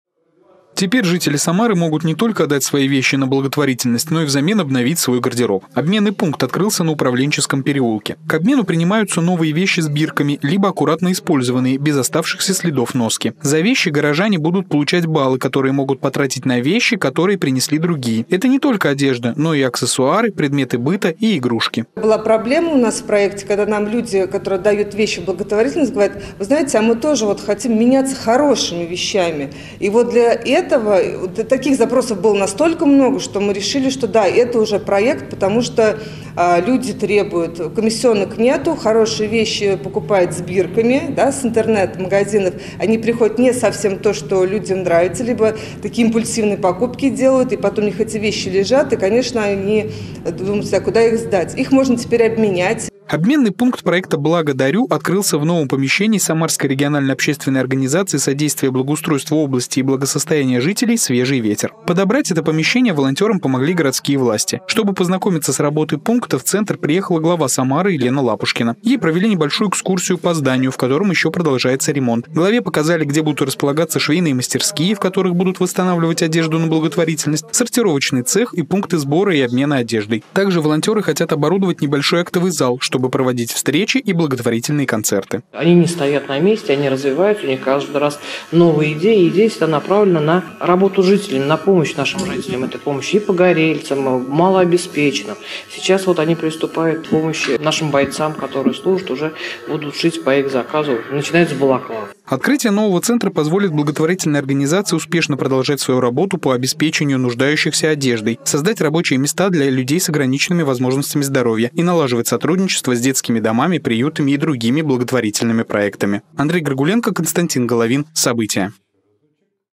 В Самаре открылся обменный пункт благотворительного проекта "БлагоДарю"
Самарцы могут обновить свой гардероб, сдав вещи, которые им не подходят. В Управленческом переулке открылся обменный пункт одежды проекта "БлагоДарю". Здесь люди могут не только сдать вещи на благотворительность, но и подобрать что-то для себя. Обменный пункт находится в новом помещении самарской организации "Свежий ветер". Что ещё здесь появится после ремонта, волонтёры показали главе города Елене Лапушкиной. Подробнее - Андрей Горгуленко.